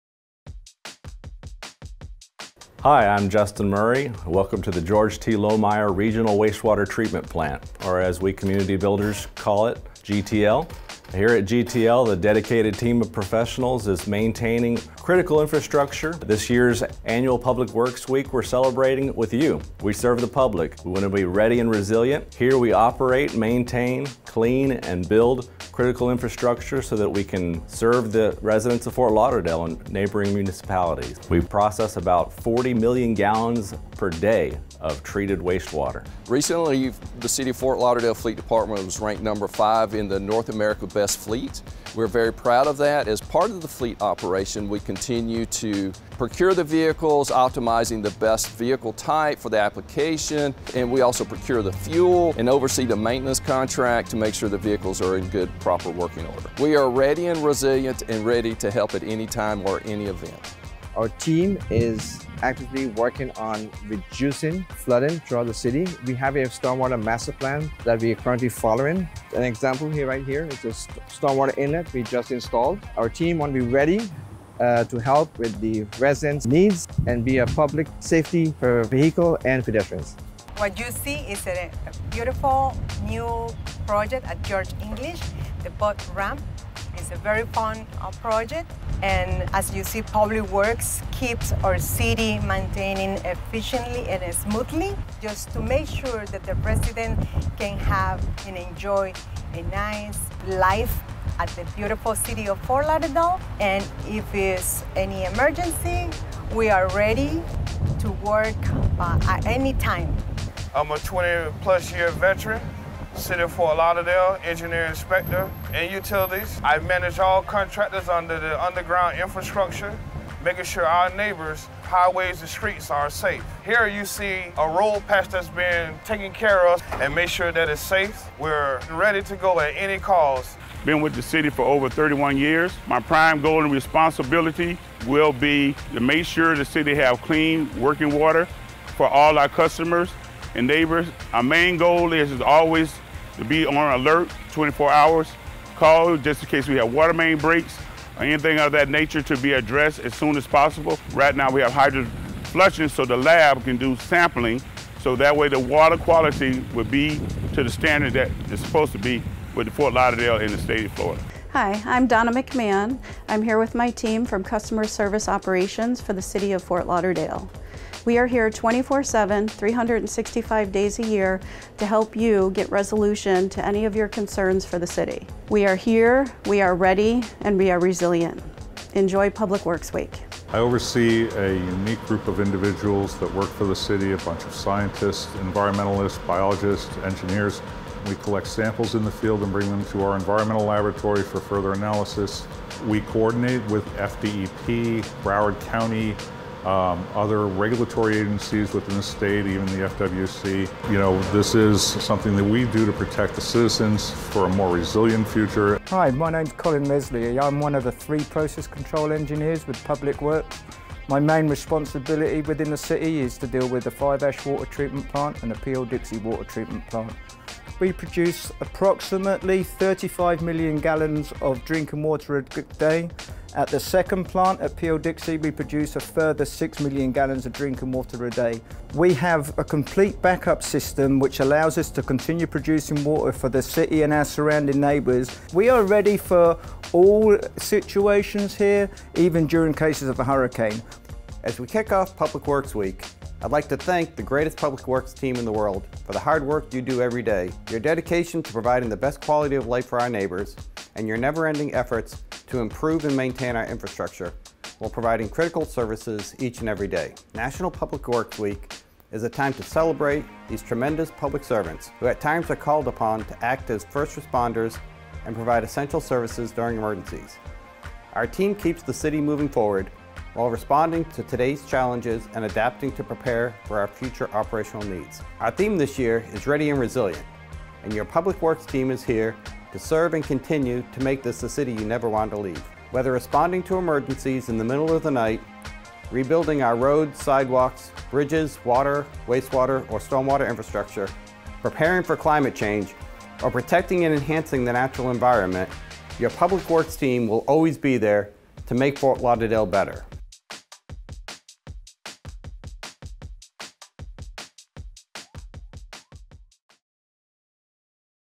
National Public Works Week 2022
Take a trip around the City to meet a few Public Works employees and learn about what they do and how we are ready and resilient!

00:02 Justin Murray - Wastewater Facilities Manager
00:36 Sandy Leonard - Fleet Program Manager
01:01 Sayd Hussain - Stormwater Engineering Project Manager II
01:20 Connie Hayman - Engineering Project Manager II
01:50 Terrance Bray - Engineering Inspector II
02:10 Paul Brown - Utilities Crew Leader
02:40 Donna McMahon - Customer Service Program Manager II
03:00 Larry Teich - Environmental Compliance Supervisor
03:25 Colin Leslie - Process Control Engineer
04:01 Alan Dodd, P.E. - Public Works Director